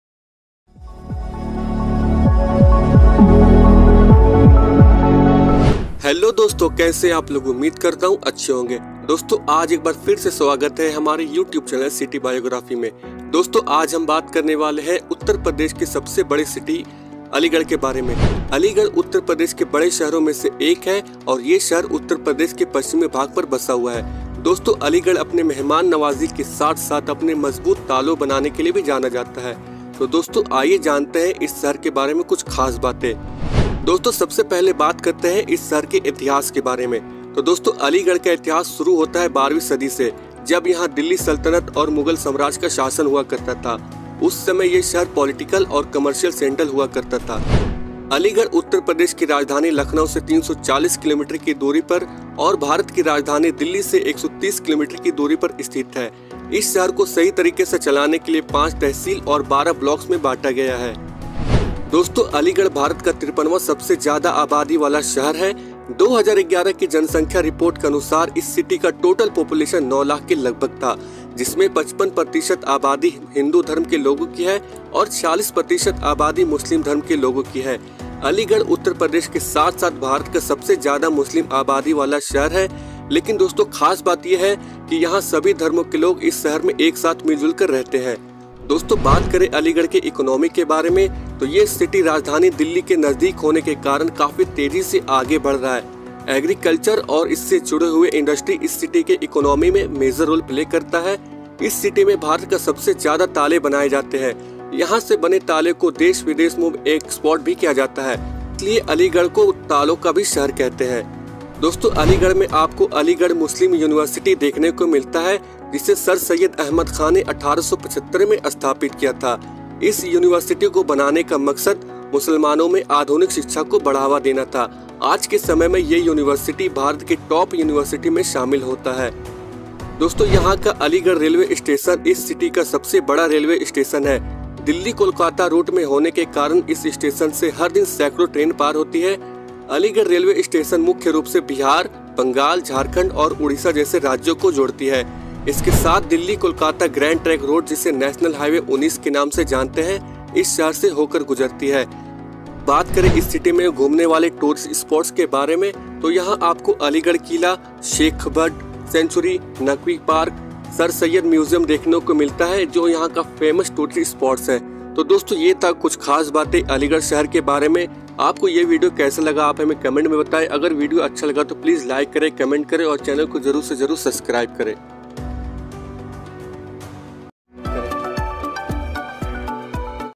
Aligarh City Tour 2023 | View & Facts | Famous For Lock Industry | Tourist Spots | City Biography
Friends, Today we are going to talk about Aligarh City of Uttar Pradesh. Aligarh is one of the big cities of Uttar Pradesh and this city is situated in the western part of Uttar Pradesh. Friends, Aligarh is also known for its hospitality as well as for making its strong locks. So friends, let us know some special things about this city. The history of Aligarh begins from the 12th century when it was ruled by the Delhi Sultanate and the Mughal Empire. At that time this city used to be a political and commercial center. Aligarh is located at a distance of 340 km from Lucknow, the capital of Uttar Pradesh and 130 km from Delhi, the capital of India. This city is divided into 5 tehsils and 12 blocks to run properly.
Aligarh is the 53rd most populous city in India. According to the population report of 2011, the total population of this city was around 9 lakhs. In which 55% of the population belongs to the people of Hindu religion and 46% of the population belongs to the people of Muslim religion. Aligarh is the city with the largest Muslim population in Uttar Pradesh as well as in India.
We create video about Cities like History, Geography, Education Activity, Transportation, Tourist Palce, Industries etc